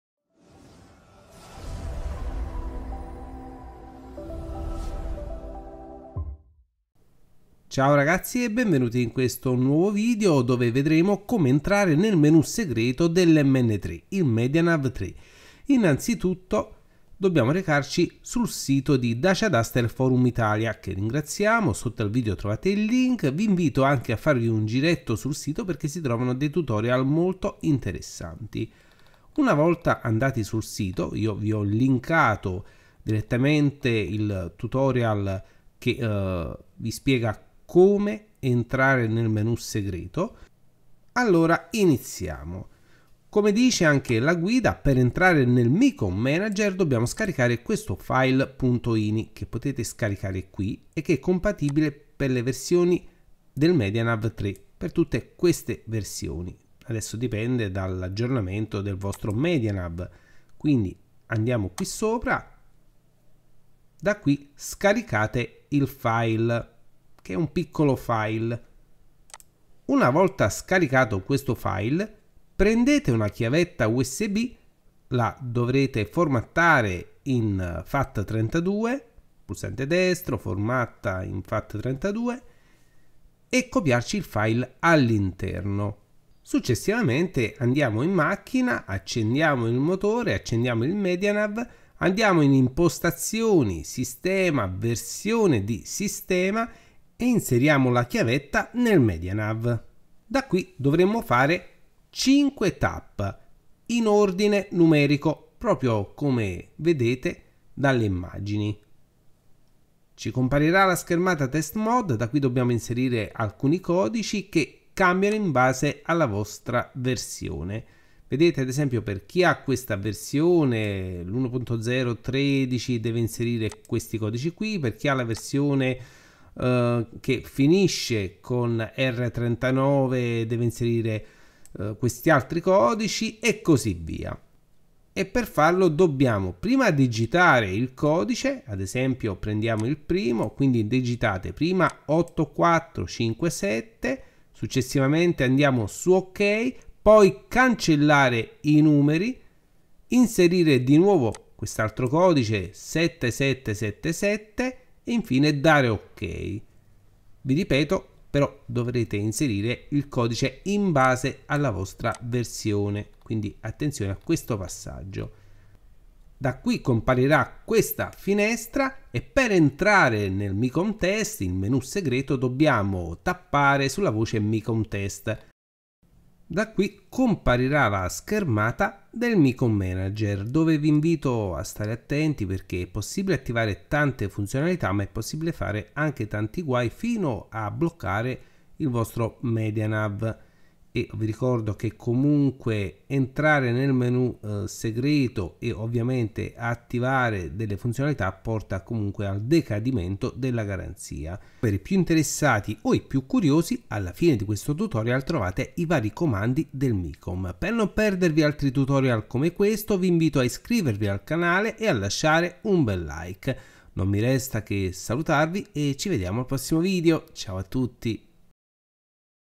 Entrare nel Menù segreto MN3 il Micom Manager Tutorial
Entrare nel Menù segreto MN3 il Micom Manager del medianav 3 Tutorial qui trovi il sito e il file di sblocco del micom manager --) semplice tutorial che ti spiega come entrare nel micom magager del medianav 3